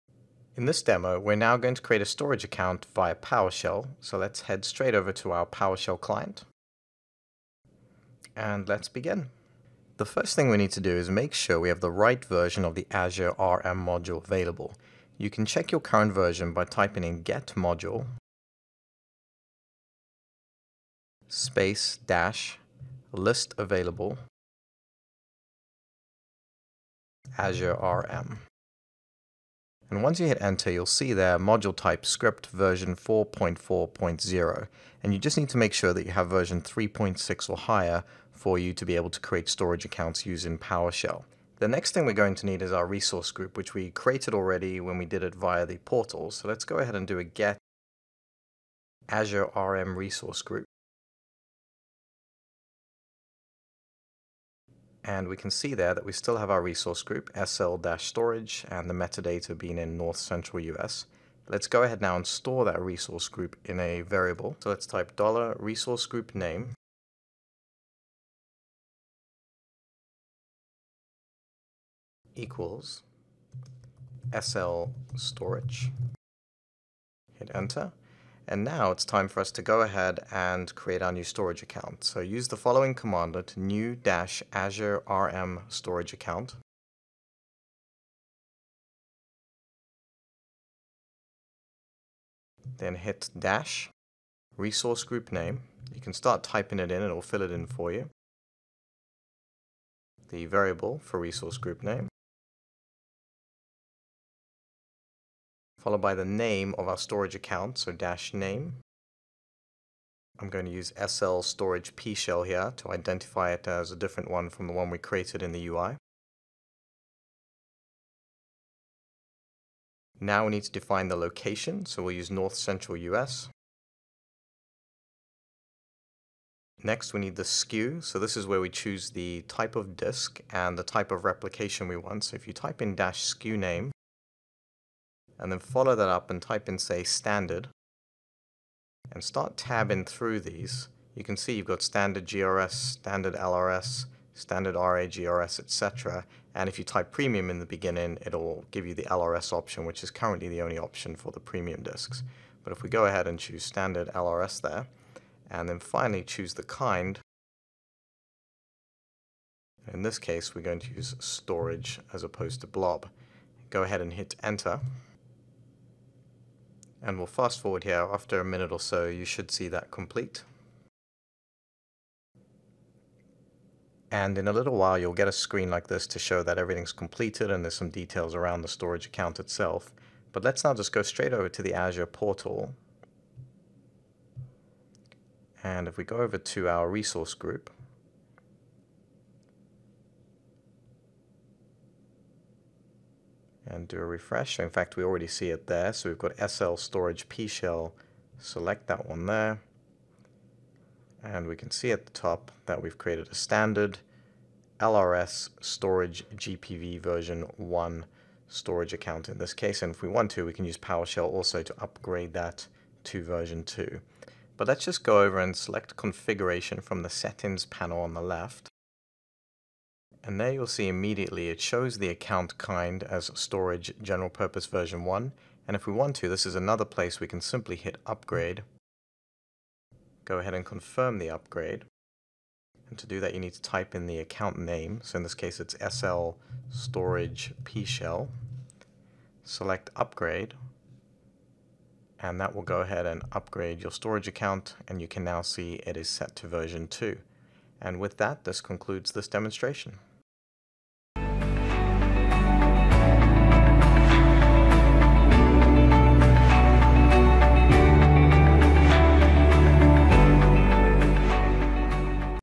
Azure Training (70-533, AZ-100) - Deploy Azure Storage Account via PowerShell
This demonstration will guide you through how to deploy an Azure Storage Account via PowerShell. This module is part of the Azure 70-533 and AZ-100 Certification Track.